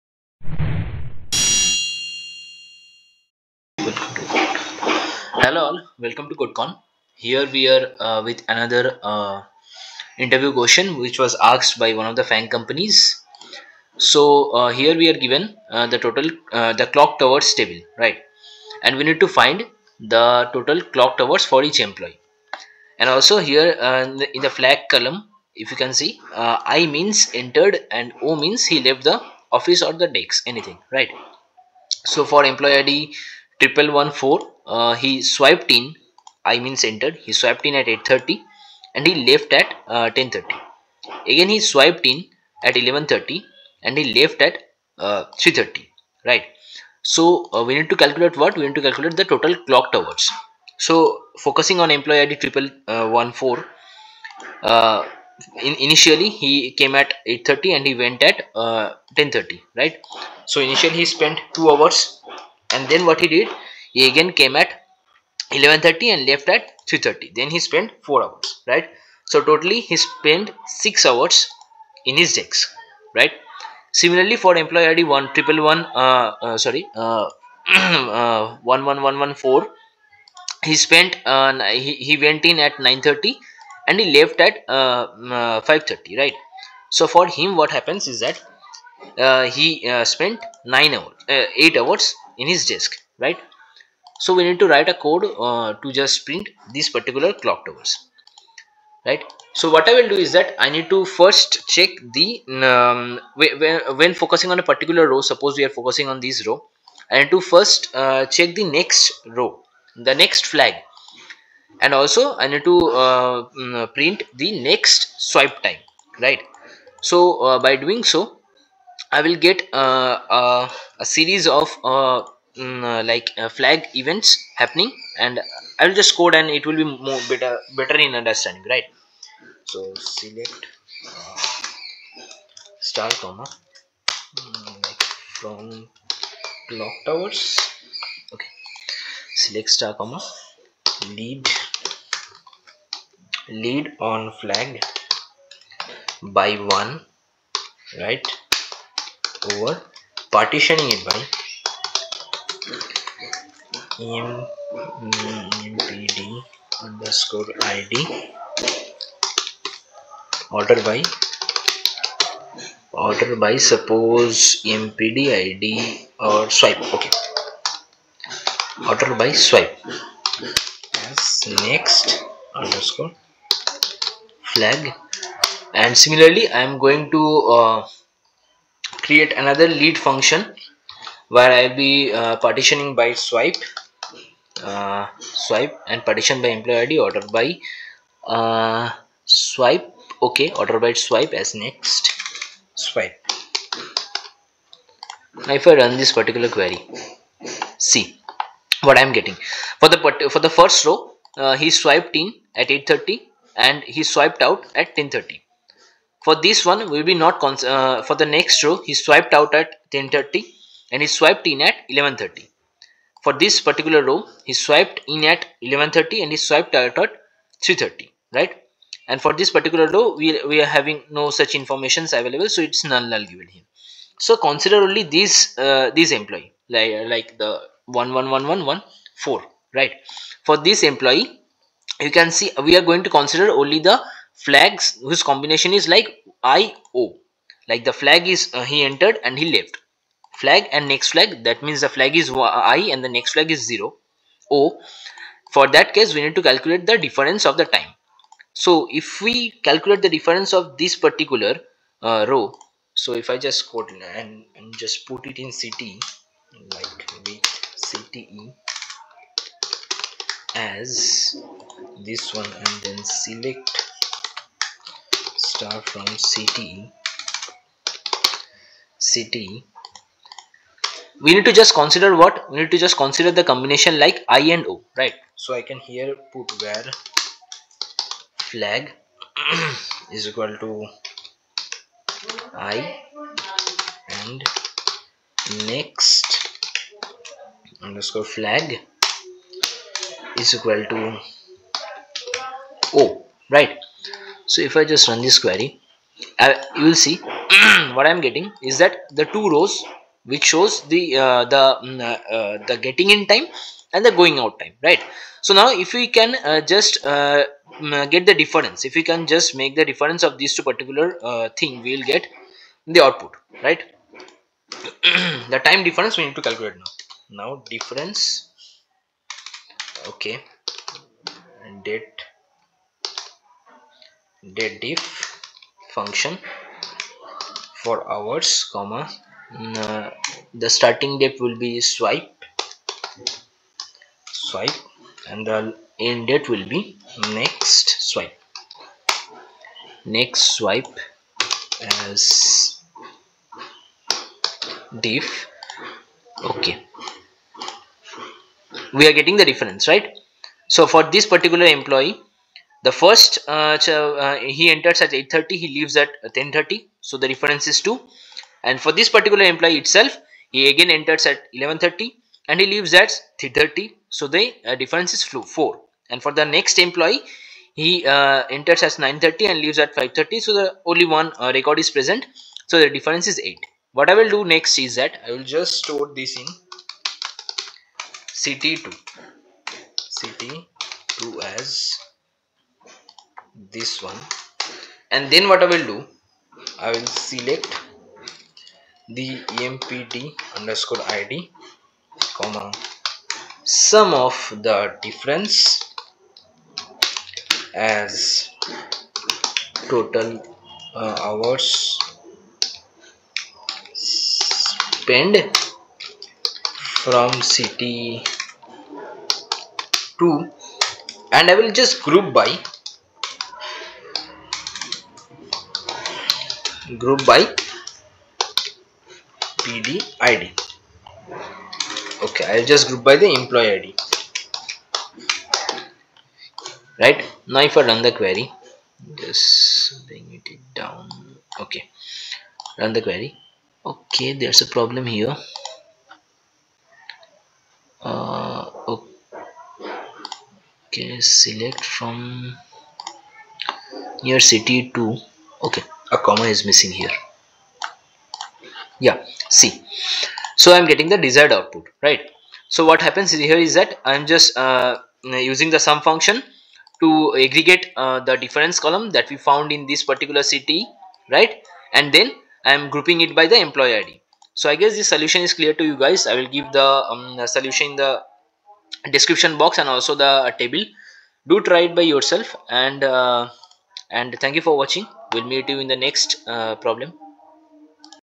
FAANG SQL Interview Question | SQL Intermediate Question 13
Hey guys, in this video i am solving a FAANG Interview question.
Do try it by yourself and comment bellow your answers.

DDL Commands :-
create table clocked_hours(
empd_id int,
swipe time,
flag char)

insert into clocked_hours values
(11114,'08:30','I'),
(11114,'10:30','O'),
(11114,'11:30','I'),
(11114,'15:30','O'),
(11115,'09:30','I'),
(11115,'17:30','O');